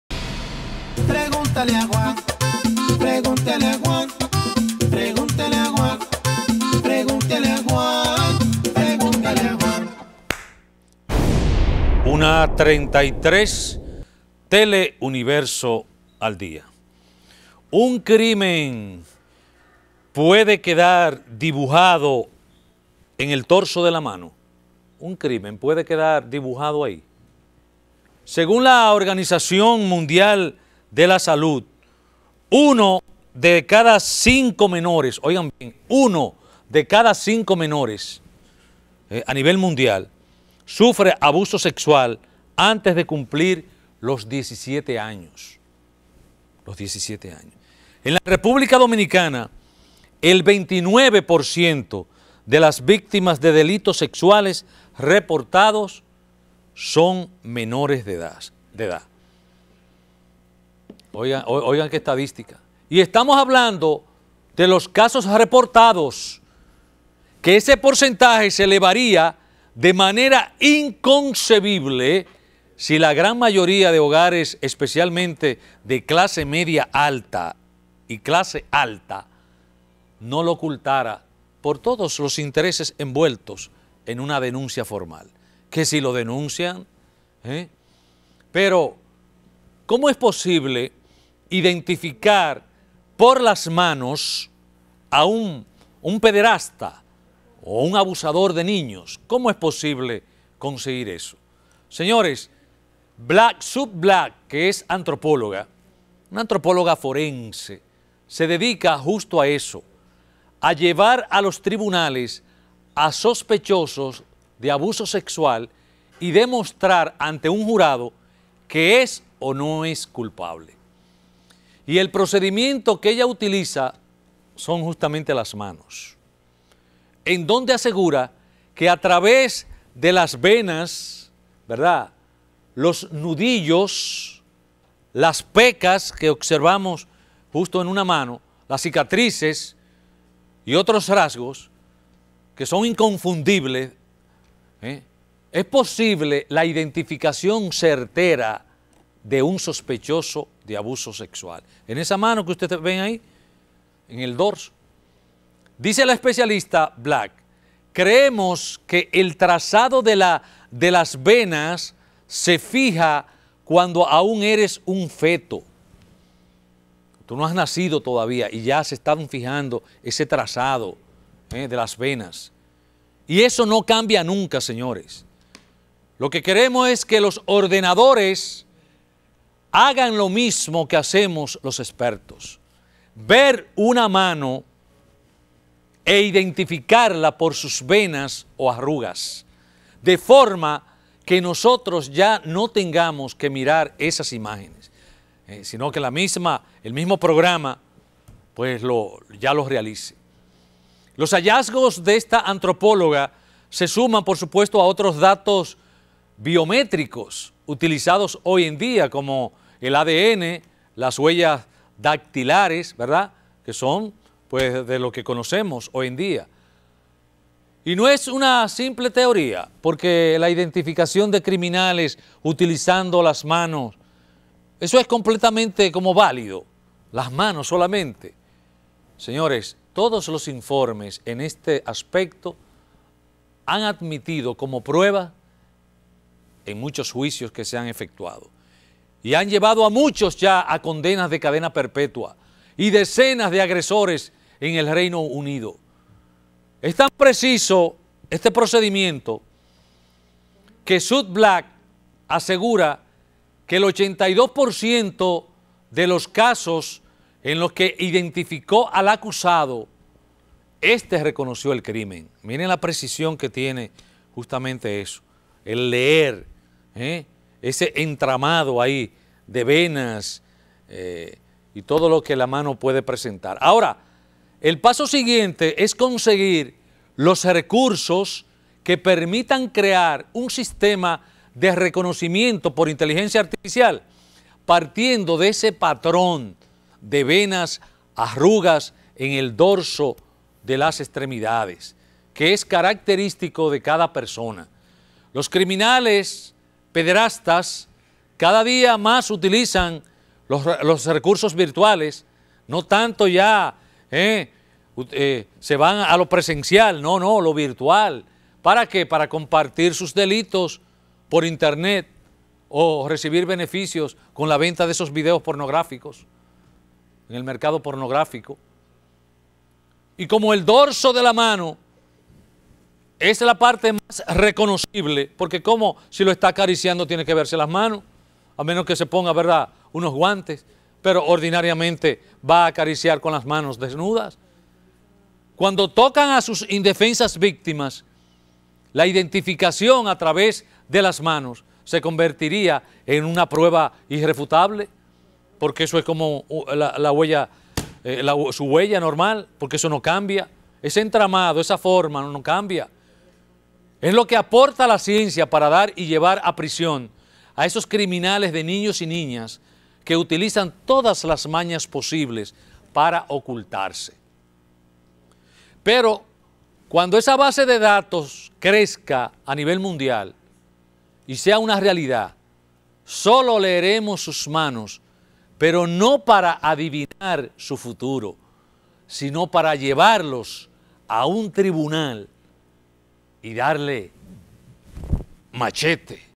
El crimen dibujado en su mano
Según la Organización Mundial de la Salud, uno de cada cinco menores sufre abuso sexual antes de cumplir los 17 años.

En la República Dominicana el 29% de las víctimas de delitos sexuales reportados son menores de edad.

Y estamos hablando de los casos reportados, que ese porcentaje se elevaría de manera inconcebible si la gran mayoría de hogares, especialmente de clase media alta y alta, no lo ocultara por todos los intereses envueltos en una denuncia formal.

Pero, ¿como es posible identificar por las manos a un pederasta o abusador de niños?

Así es, Sue Black, que es antropóloga forense se dedica justo a eso: a llevar a los tribunales a sospechosos de abuso sexual y demostrar ante un jurado que es o no es culpable. Y el procedimiento que utiliza son las manos, en donde asegura que a través de las venas, nudillos, pecas, cicatrices y otros rasgos inconfundibles de las manos, es posible la identificación certera de un sospechoso de abuso sexual.

Dice la especialista Black “creemos que el trazado de las venas se fija cuando aún eres un feto y no cambia nunca. lo que queremos es que los ordenadores hagan lo mismo que hacemos los expertos: ver una mano e identificarla por sus venas o arrugas, de forma que nosotros ya no tengamos que mirar esas imágenes”